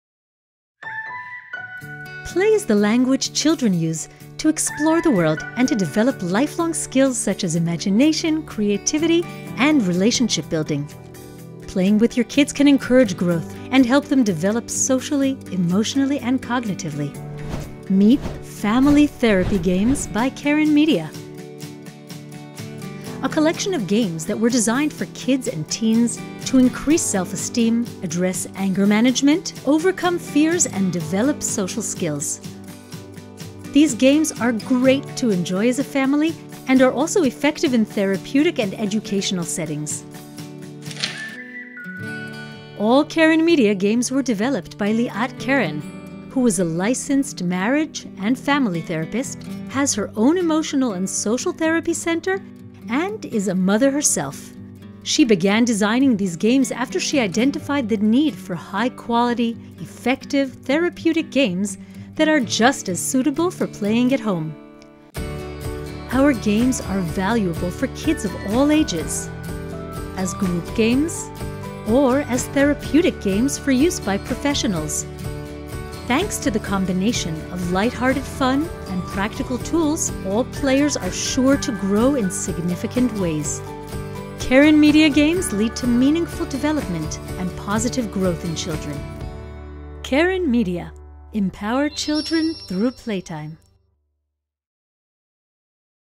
Keren Media Introduction
At Keren Media our mission is to empower children through playtime. We have developed a series of fun, therapeutic and effective games that help kids learn and nurture valuable emotional skills.   Uniquely created, Keren Media games are designed for therapists and other professionals who work with kids through the challenges of developing social skills; while also helping parents, with no formal therapeutic training, take a more active role in helping their kids grow emotionally.  The games are graphically enticing, have easy-to-understand instructions and can be easily adapted and modified to individual children’s needs.